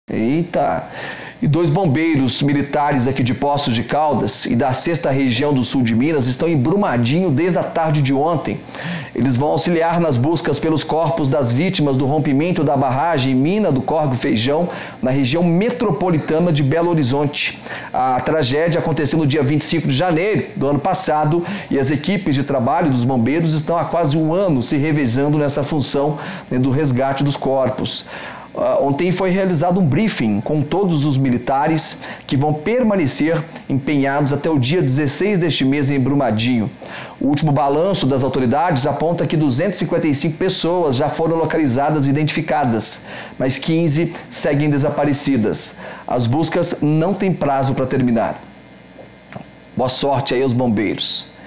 BOMBEIROS EM BRUMADINHO
BOMBEIROS DE POÇOS E DO SUL DE MINAS SEGUEM EM BRUMADINHO

Militares do Corpo de Bombeiros de Poços de Caldas e da região Sul de Minas seguiram para Brumadinho na tarde desta última quarta-feira (8), eles seguem auxiliando nas buscas por corpos de vítimas da tragédia do dia 25 de Janeiro de 2019, com o rompimento da barragem da mina Córrego do Feijão.